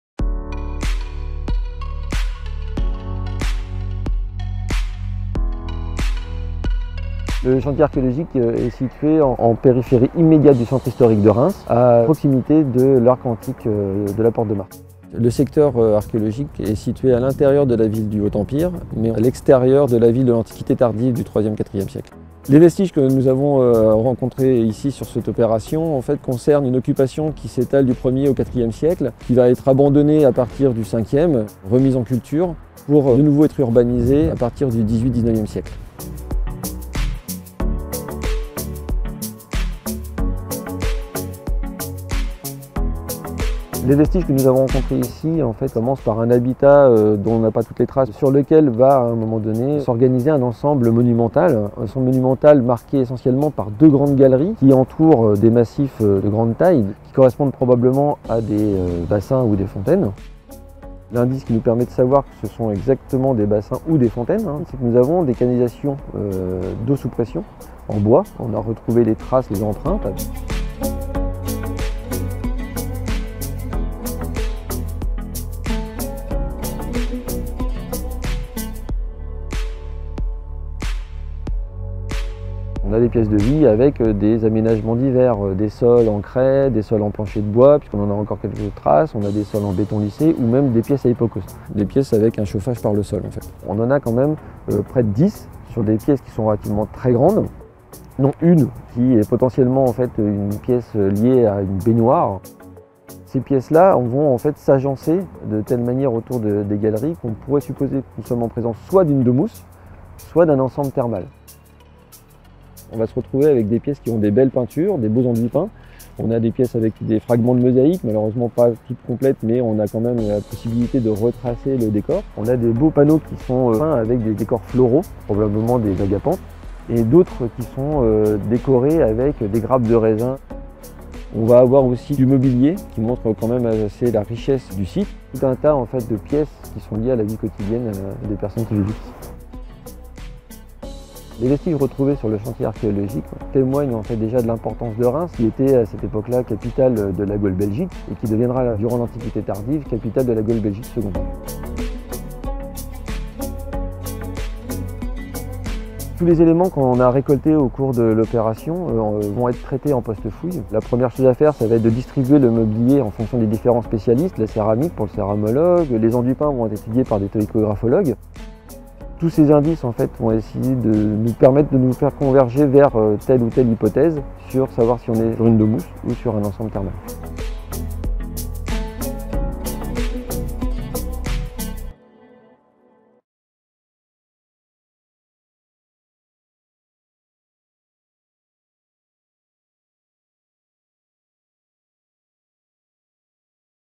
Un site antique monumental à Reims (Marne)
Des archéologues de l'Inrap ont mis au jour un ensemble monumental des IIe et IIIe siècles à Reims (Marne), a proximité de la porte de Mars, vestige romain emblématique de la ville.
De nombreuses pièces dotées d'un systèmes de chauffage par le sol (hypocauste) ont été découvertes ainsi qu'un ensemble d'enduits peints richement décorés.